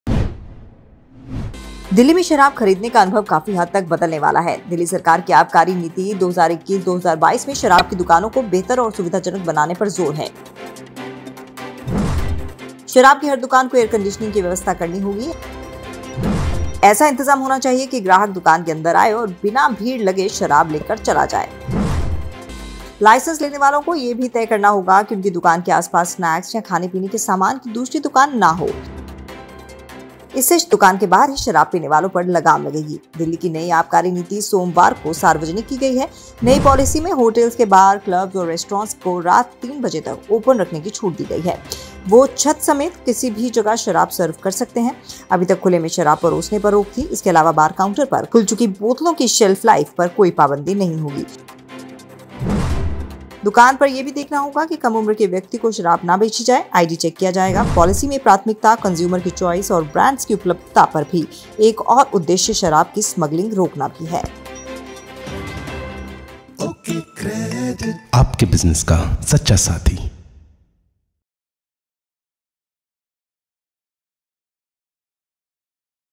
Delhi liquor policy: नई आबकारी नीति, बार क्लब में रात 3 बजे तक मिलेगी मदिरा | OkCredit
दिल्‍ली में शराब खरीदने का अनुभव काफी हद तक बदलने वाला है। दिल्ली सरकार की आबकारी नीति 2021-22 में शराब की दुकानों को बेहतर और सुविधाजनक बनाने पर जोर है। शराब की हर दुकान को एयर-कंडीशनिंग की व्‍यवस्‍था करनी होगी। ऐसा इंतजाम होना चाहिए कि ग्राहक दुकान के अंदर आए, और बिना भीड़ लगे शराब लेकर चला जाए।
लाइसेंस लेने वालों को यह भी तय करना होगा कि उनकी दुकान के आसपास स्‍नैक्‍स या खाने-पीने के सामान की दूसरी दुकान न हो। इससे दुकान के बाहर ही शराब पीने वालों पर लगाम लगेगी। दिल्‍ली की नई आबकारी नीति सोमवार को सार्वजनिक की गई है। नई पॉलिसी में होटलों के बार, क्‍लब्‍स और रेस्‍टोरेंट्स को रात 3 बजे तक ओपन रखने की छूट दी गई है।
वे छत समेत किसी भी जगह शराब सर्व कर सकेंगे। अभी तक खुले में शराब परोसने पर रोक थी। इसके अलावा बार काउंटर पर खुल चुकीं बोतलों की शेल्‍फ लाइफ पर कोई पाबंदी नहीं होगी। दुकान पर यह भी देखना होगा कि कम उम्र के व्‍यक्ति को शराब न बेची जाएगी। आईडी चेक क‍िया जाएगा। पॉलिसी में प्राथमिकता, कंज्‍यूमर की चॉइस और ब्रैंड्स की उपलब्‍धता पर भी है और एक उद्देश्य शराब की स्‍मगलिंग रोकना भी है।